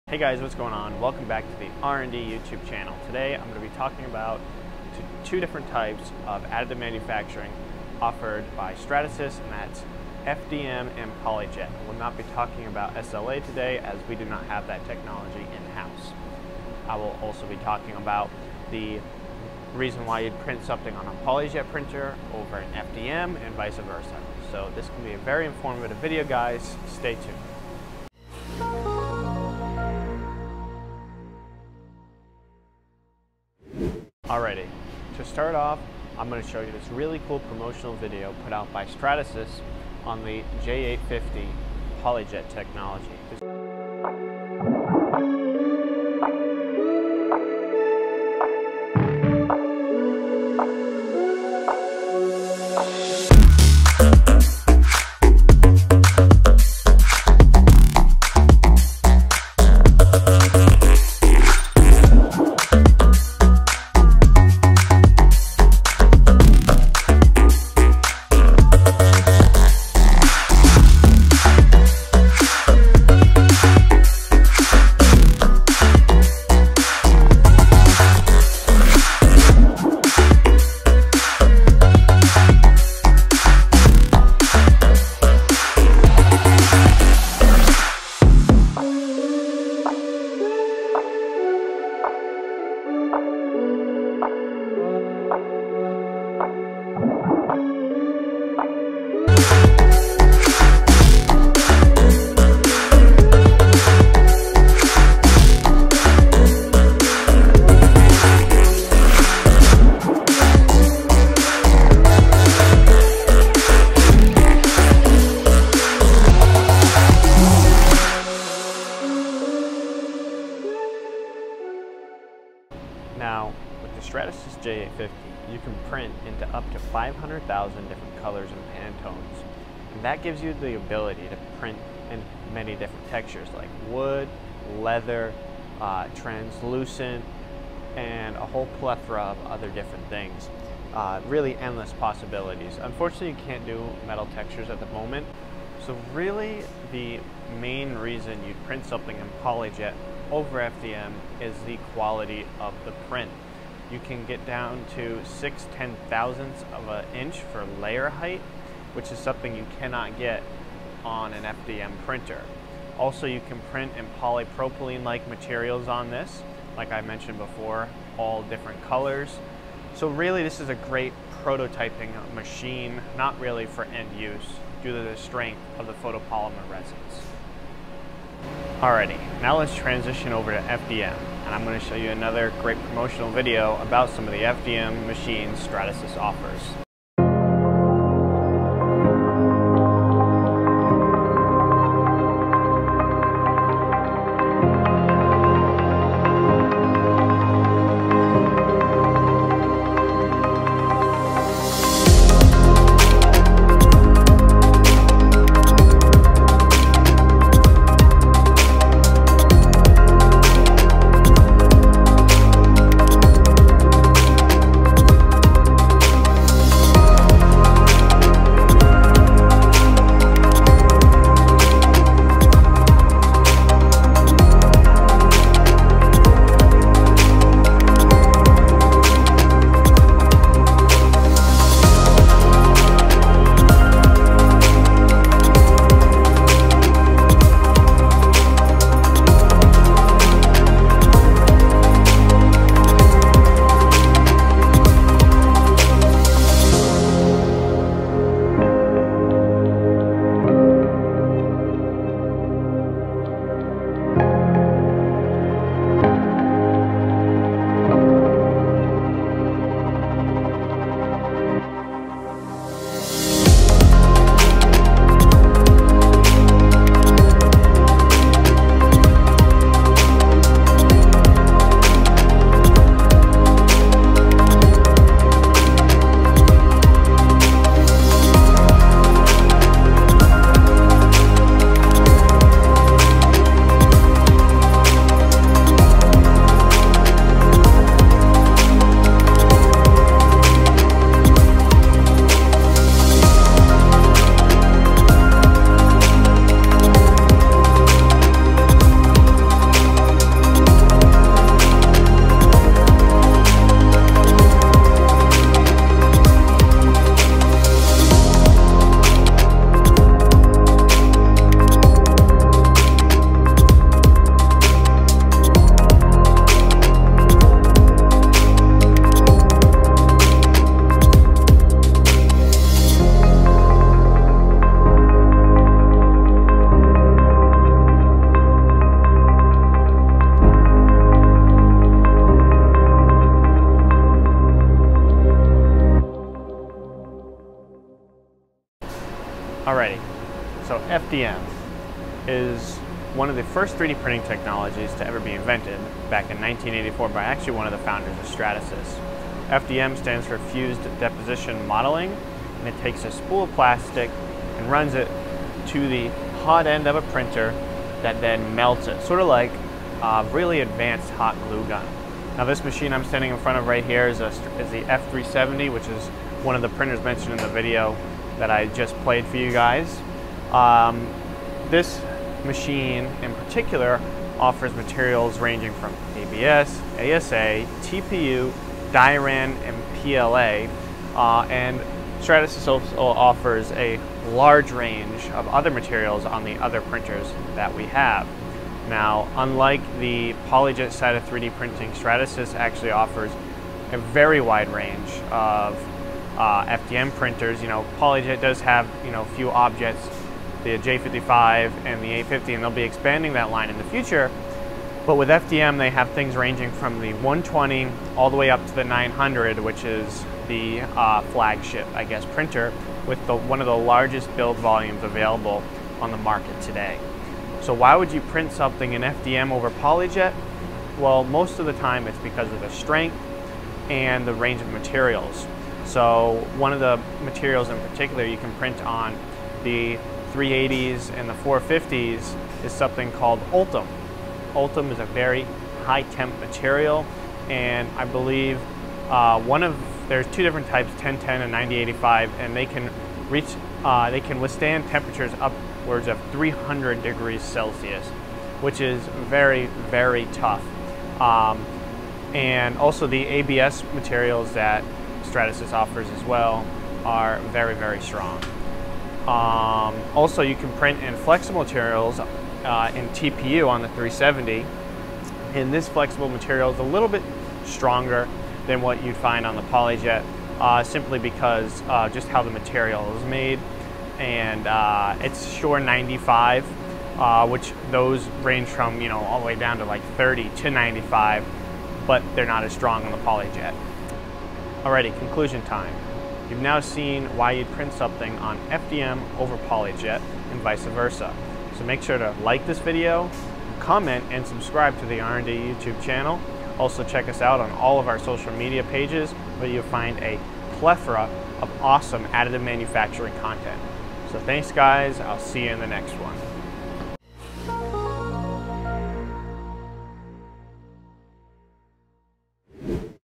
FDM vs PolyJet 3D Printing Breakdown
Stratasys offers two main type of 3D printing technologies. Both offer benefits over the other. So which would work best for you? PolyJet or FDM? Time to find out!

Rnd-tech.com